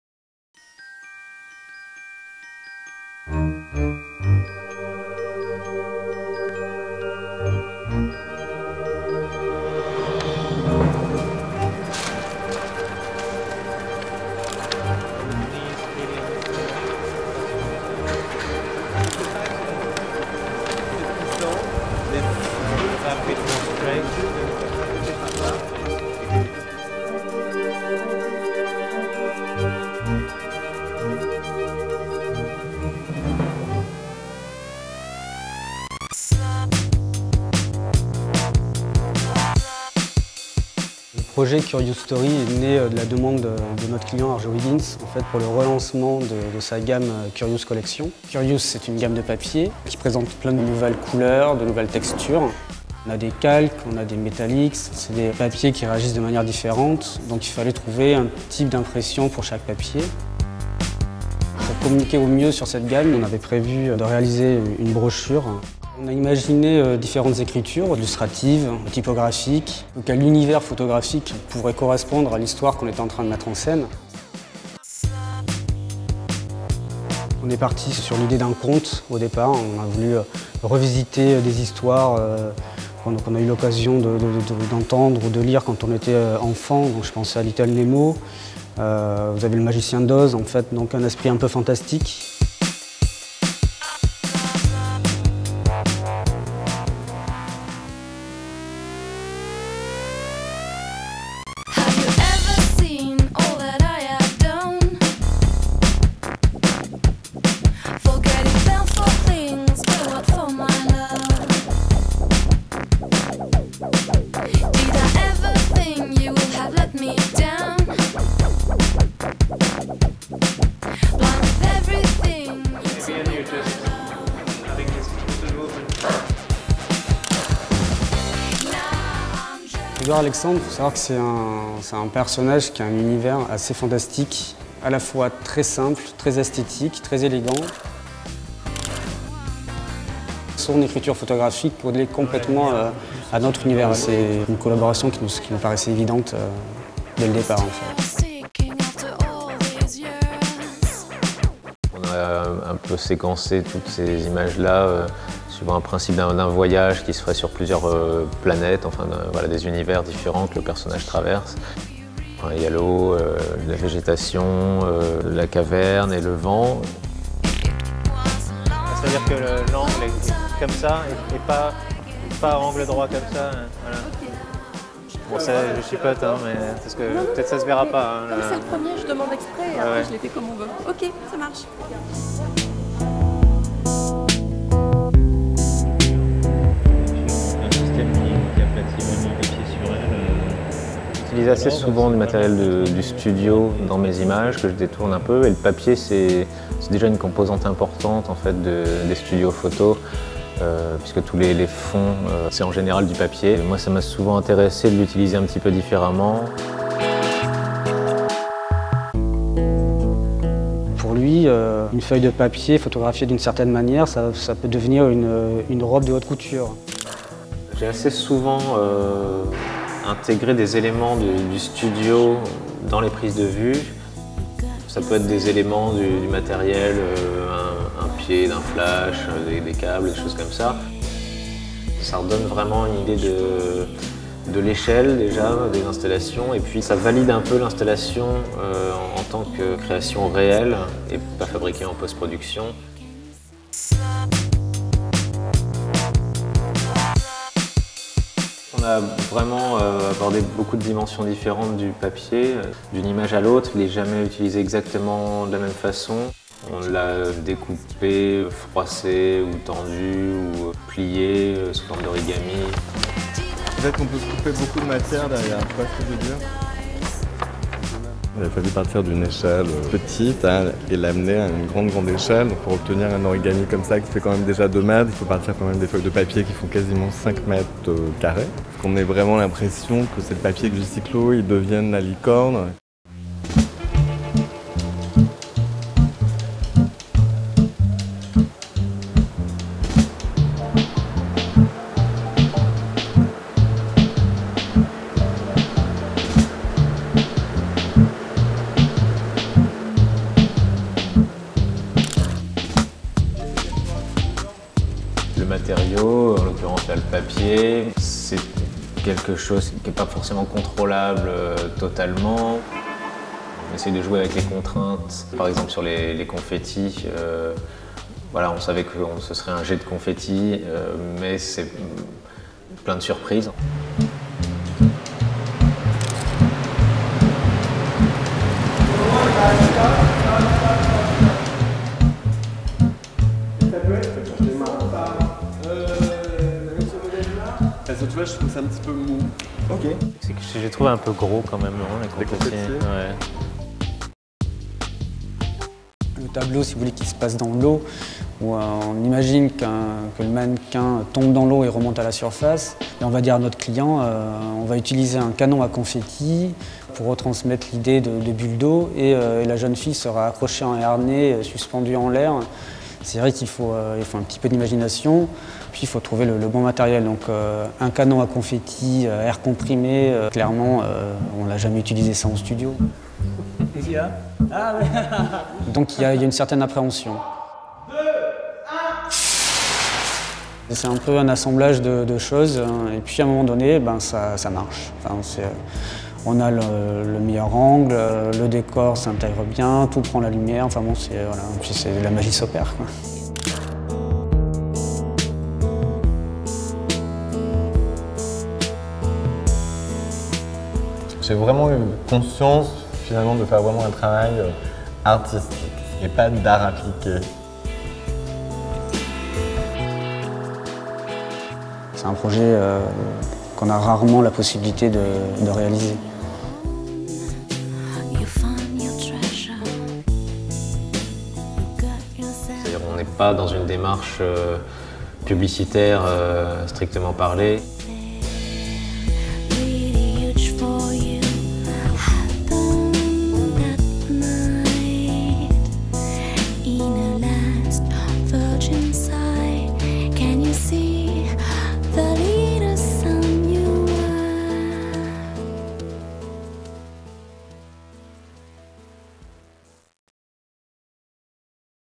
The Making of The Curious Story
Behind the scenes of the making of The Curious Story - created for the relaunch of Arjowiggins Curious Collection range.